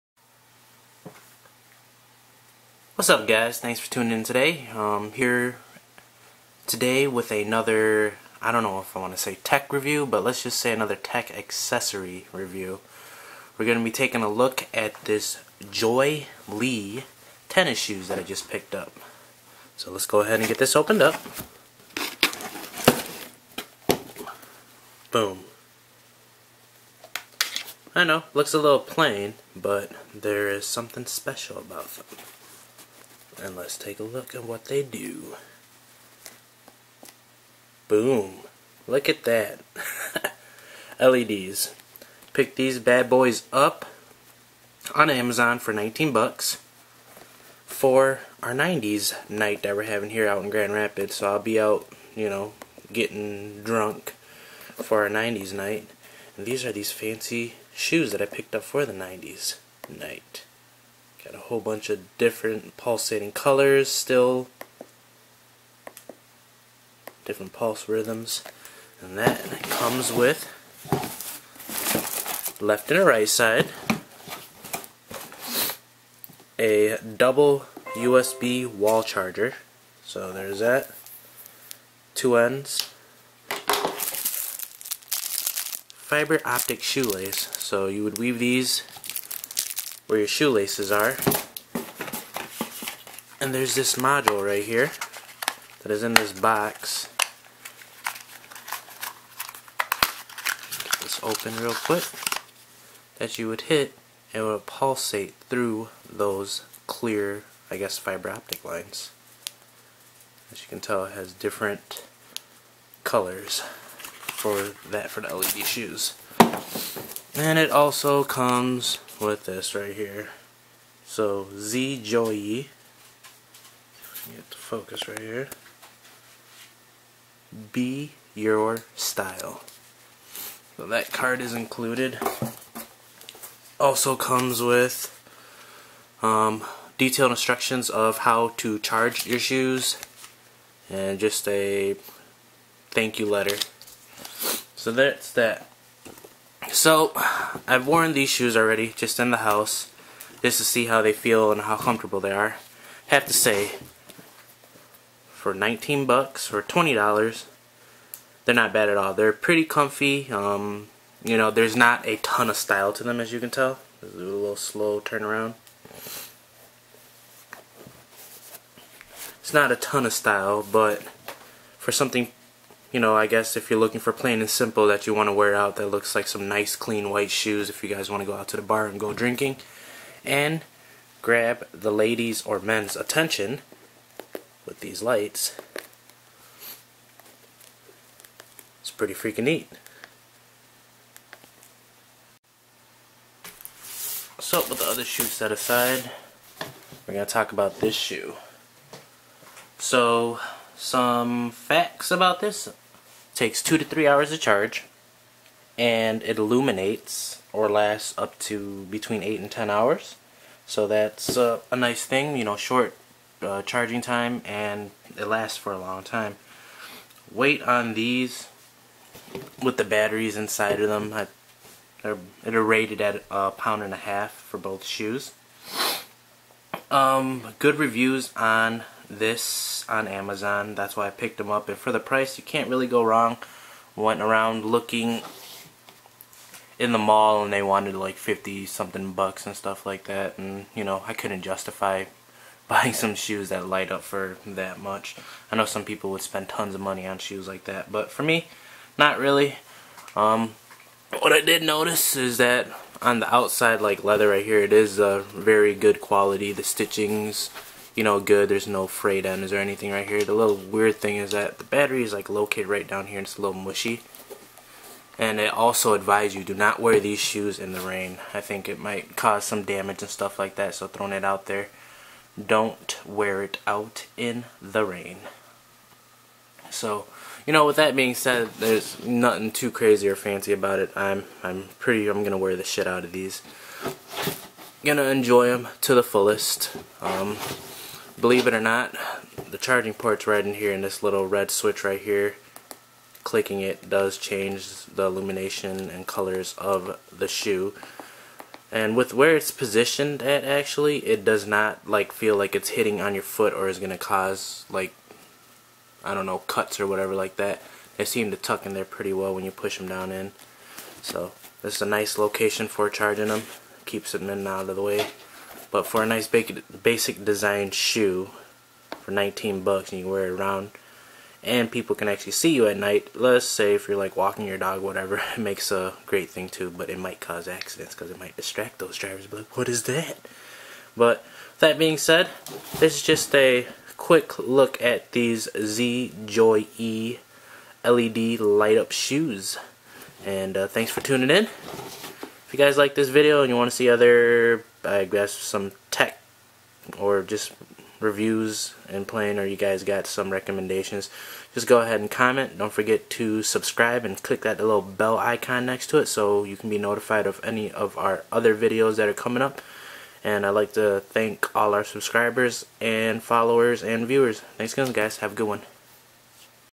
JOYEE LIGHT UP SHOES
In this video I will be unboxing some fancy light up shoes and showing how they work.